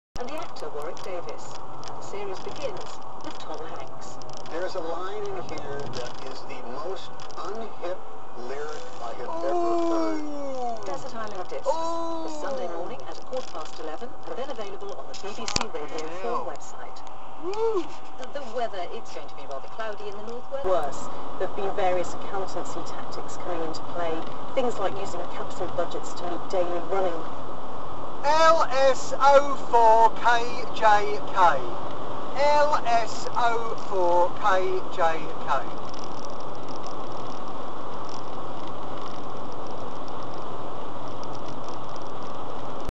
LS04 KJK : Typical Kentish Lane Hog (Travelling 62MPH in the A249's overtaking lane)
The driver of this vehicle (LS04 KJK) was an elderly woman.  I normally NEVER, EVER, undertake as to do so simply compounds the pig-ignorant and bad driving on Kent's roads.

However... I was in the driving lane and, since I was stuck behind her (and she was pootling along doing 62MPH, oblivious to the dangerous chaos she was causing behind her) and I was being flashed from the aggressively driven 4x4 car behind me...and since in 'her' overtaking lane there was also a queue of cars behind her (I estimate the queue of traffic to have been well over a half a mile of cars being held up behind her 62MPH jaunt), I decided that I would risk undertaking her simply for safety reasons (I stand by my decision, made on safety grounds, as the numerous other motorists that were being 'held up' were evidently getting dangerously agitated by her unabashed lane-hogging).